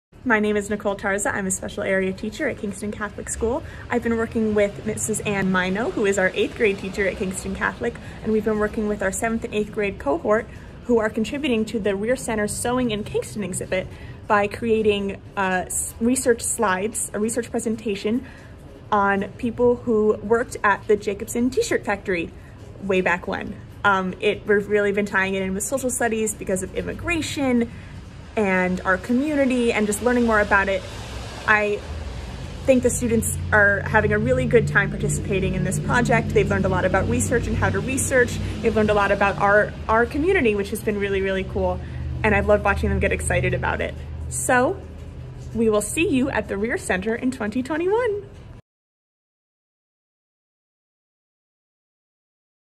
Nicole Tarcza: Grow our Gallery, Build our Bathrooms!
Nicole Tarcza, teacher at Kingston Catholic School, worked with Mrs. Ann Mino and her students in collaboration with the Reher Center and the Ulster County Archives this fall to research the backgrounds of girls and women who worked at the F. Jacobson and Sons Shirt Factory in 1918.

At the end of November, they passed their research along to Mrs. Stephanie Baird, whose English students at Rhinebeck High will be composing diary entries from each of those girls'/womens' perspectives. In the spring, Ulster BOCES sewing students will use these diary entries to alter reproduction shirts (designed by local seamstress and pattern maker Maria Cabrera) to reflect the individuality of each of these former workers.

The final student work from all three schools will be on display at the Reher Center Courtyard as part of our Sewing in Kingston exhibition, opening in 2021.

Contribute to support programs like these!  Your dollars be matched by a generous donor until December 1st, 2020.